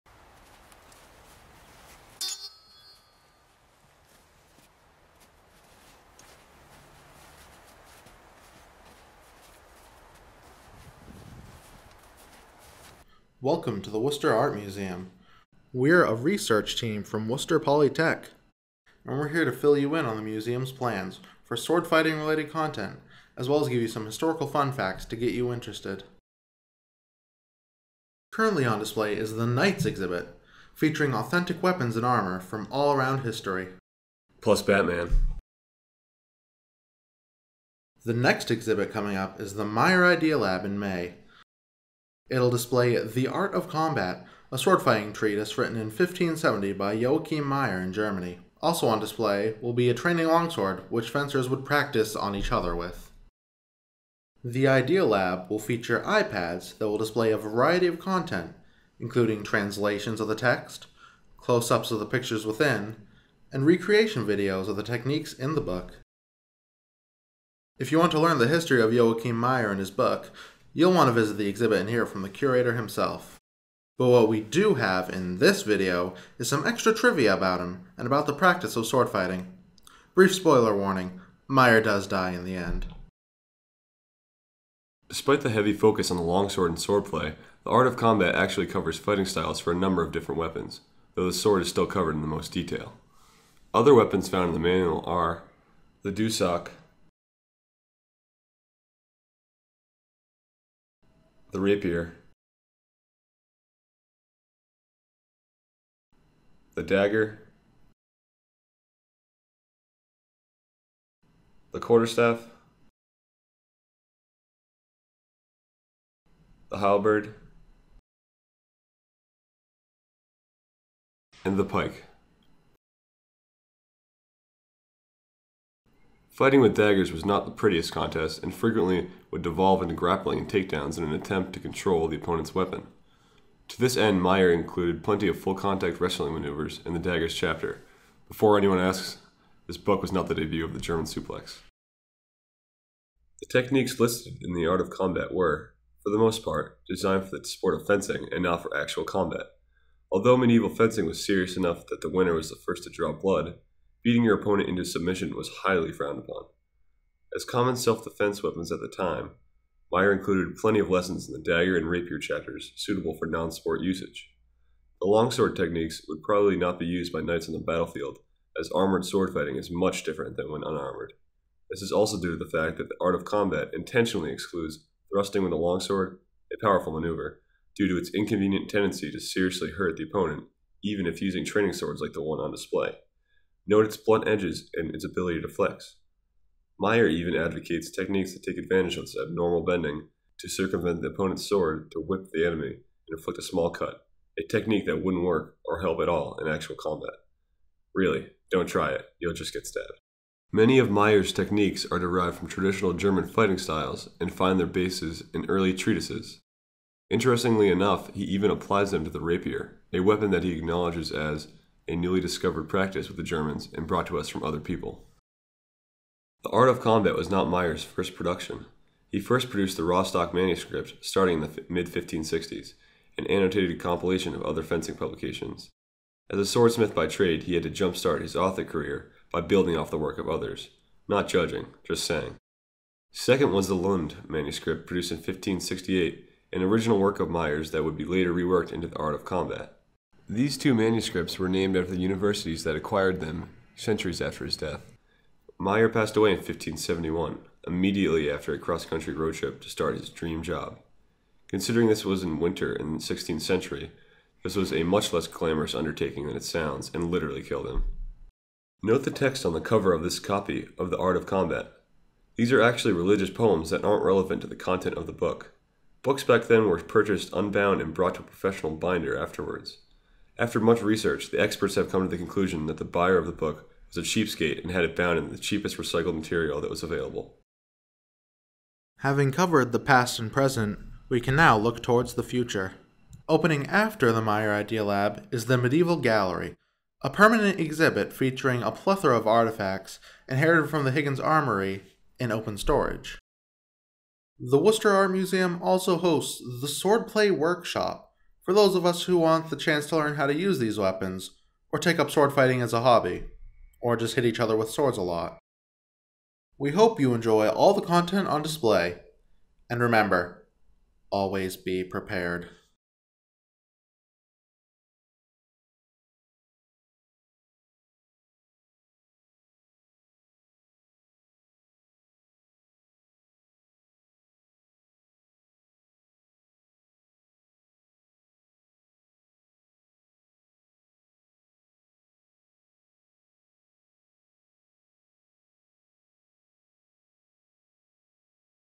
Visitor Engagement at the Worcester Art Museum
The goal of this project was to improve visitor engagement in the WAM's new medieval armory-inspired exhibits. The team gathered research, interviewed professionals, visited local museums, and performed surveys and human observation. Using the compiled data, the team provided recommendations to better engage visitors that visit the exhibits as well as an iPad app for the upcoming Idea Lab exhibit.